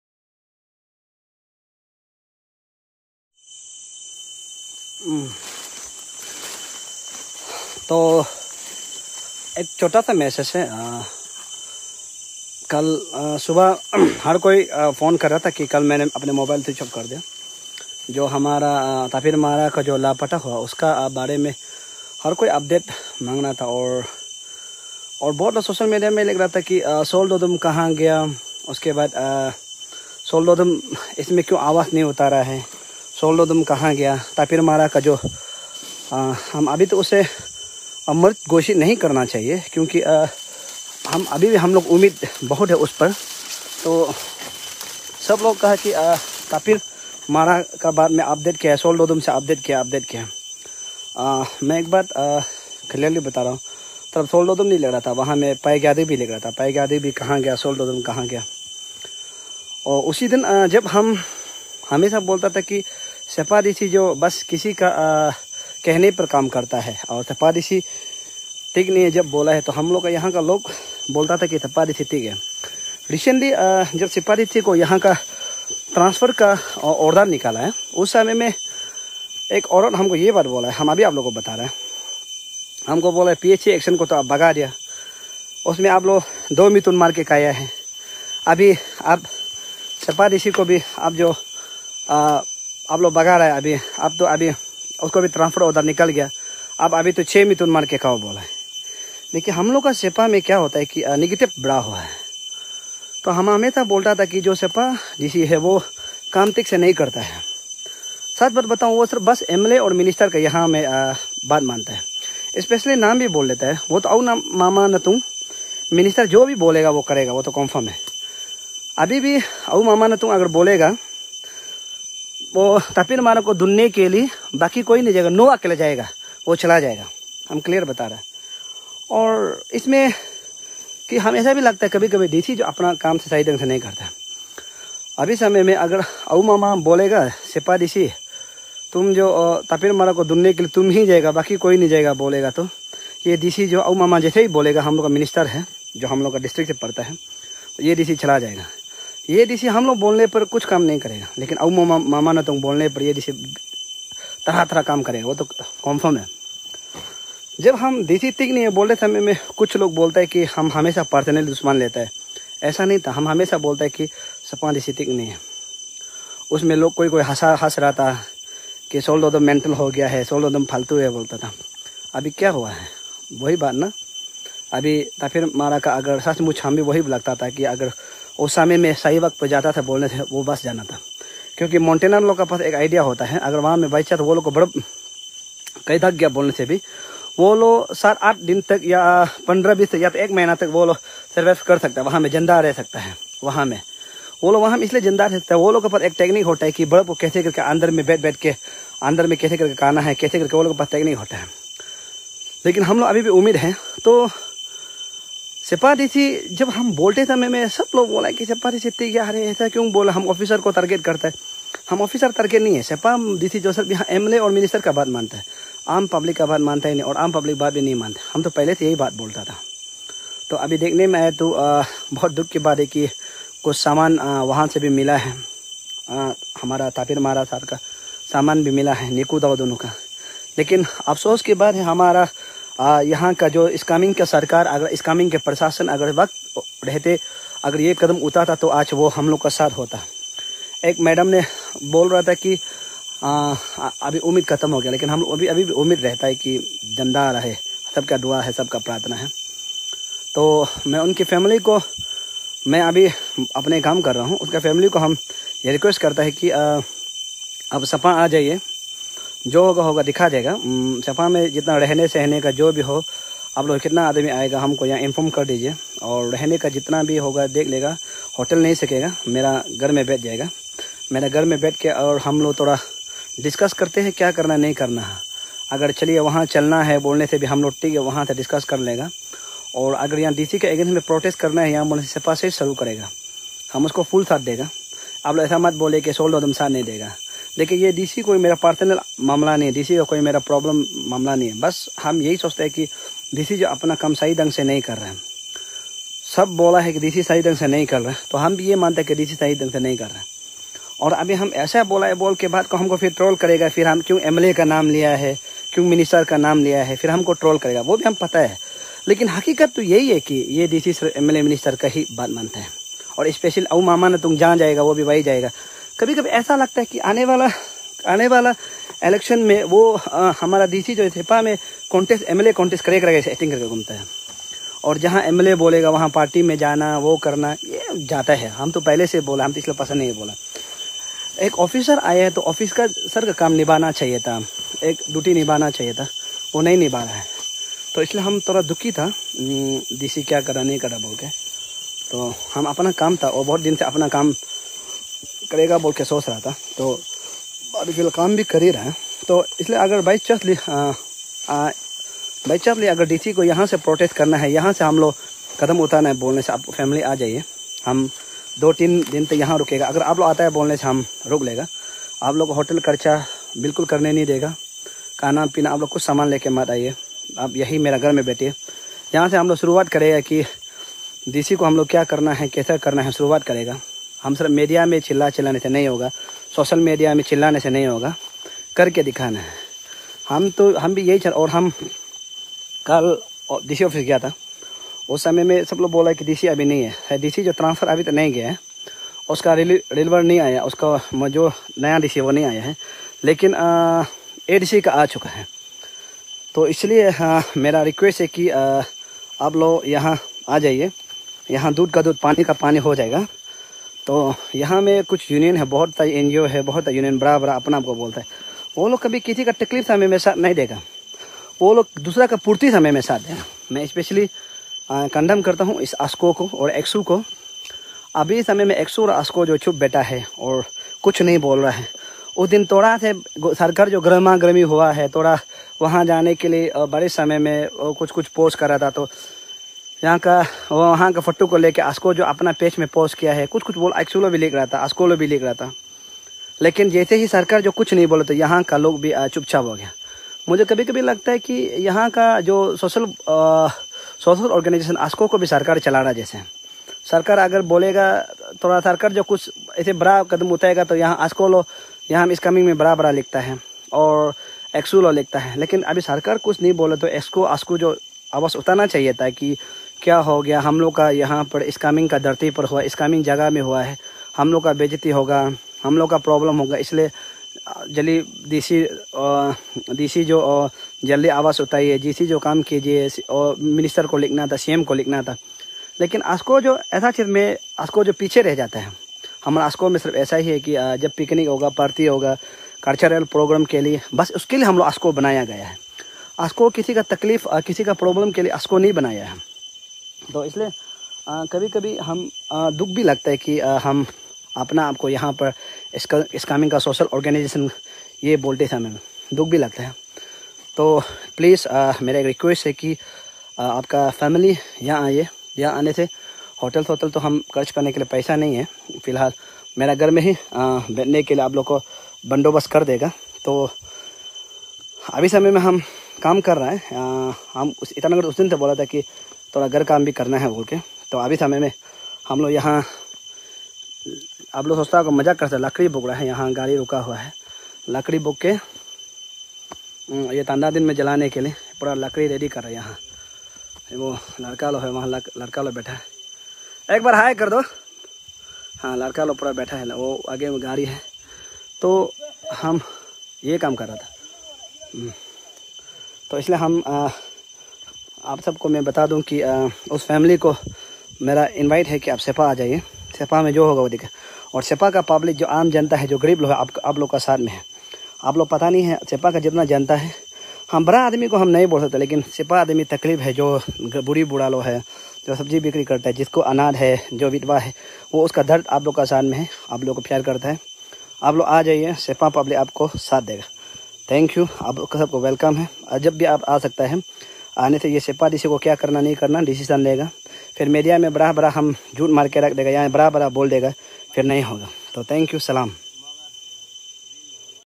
Tapi Mara missing case/ Sol Dodum lashes DC Seppa/ Delay of rescue operation#bjpgovt
Tapi Mara missing case/ Sol Dodum Statement/ Delay of rescue operation